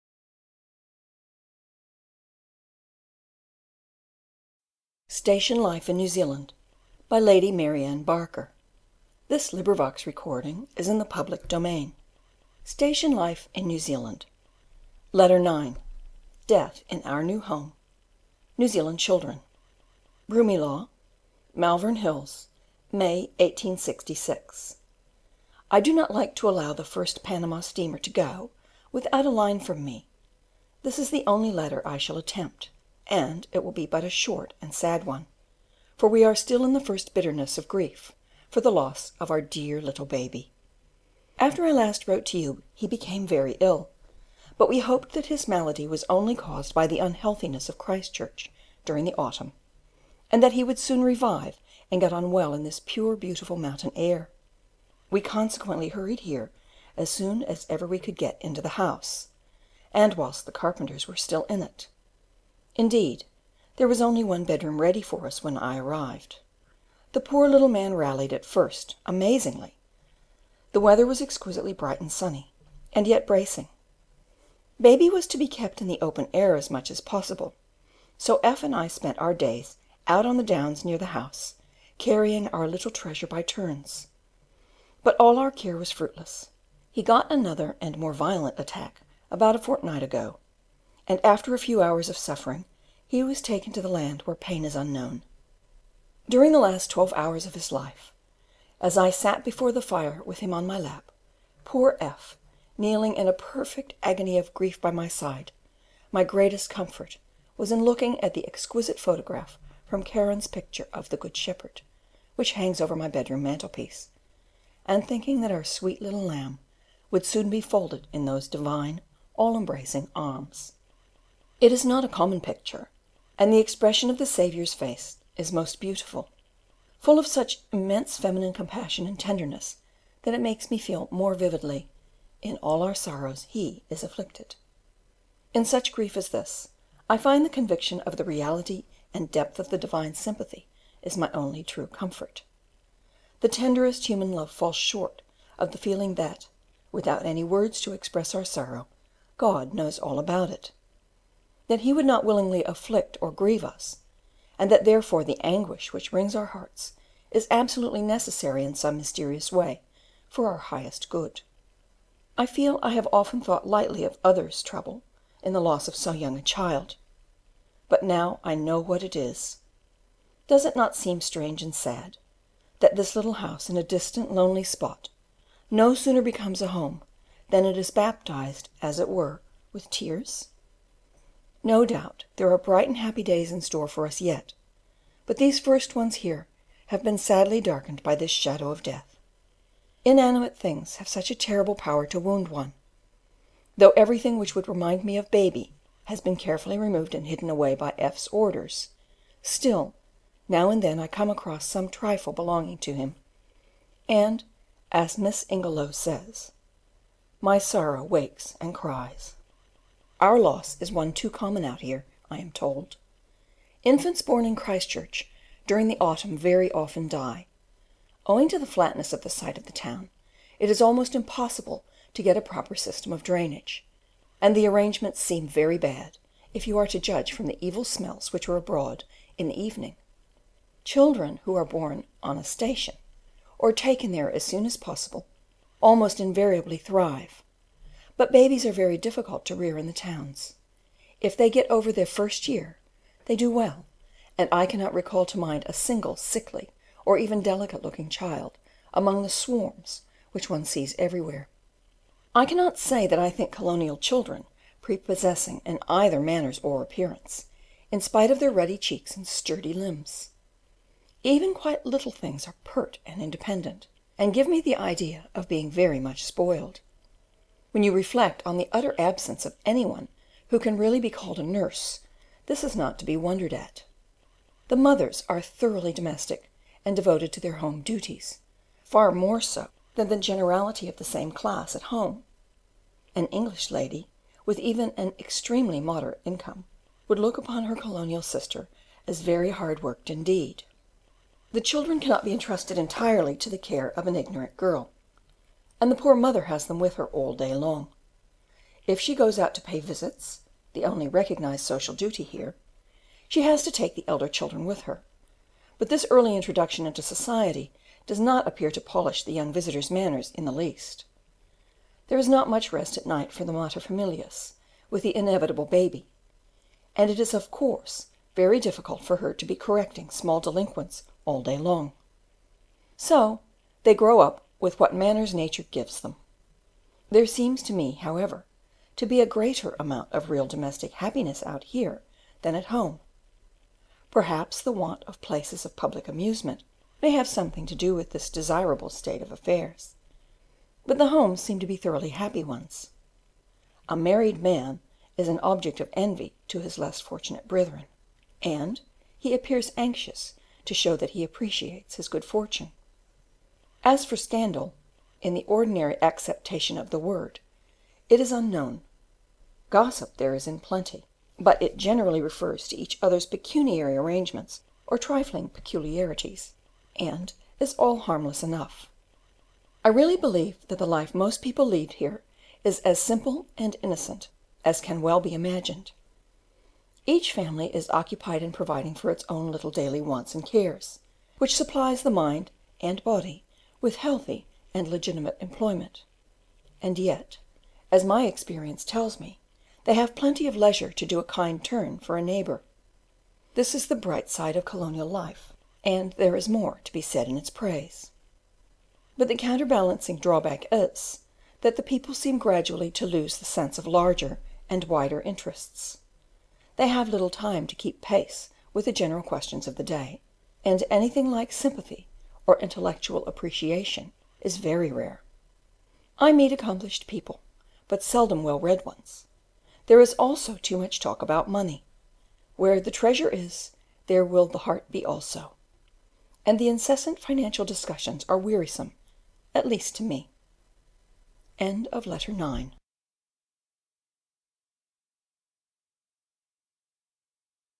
Station Life in New Zealand by Mary Anne Barker - Letter 9: Death in our new home
LibriVox recording of Station Life in New Zealand by Lady Mary Anne Barker. Read by Gail Timmerman Vaughan.

Cover picture by Charles Decimus Barraud (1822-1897), Lake Coleridge Station, 1870. Copyright expired in U.S., Canada, EU. and all countries with author's life +70 yrs laws. Cover design by Gail Timmerman-Vaughan. This design is in the public domain.